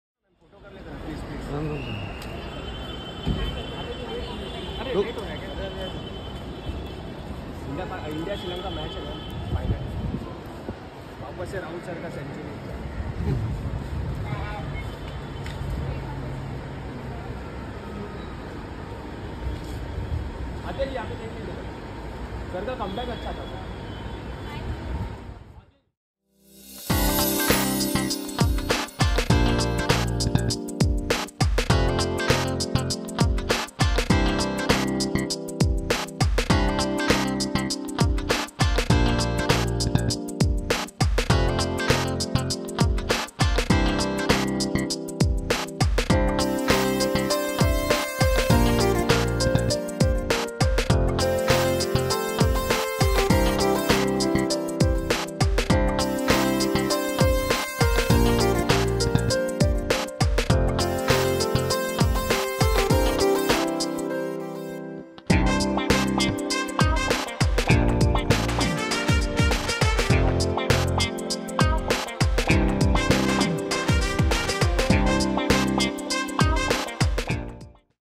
Athiya Shetty Looking Gorgeous In Traditional Avatar
Suniel Shetty's daughter Athiya Shetty was seen wearing an Indian suit at Mumbai airport.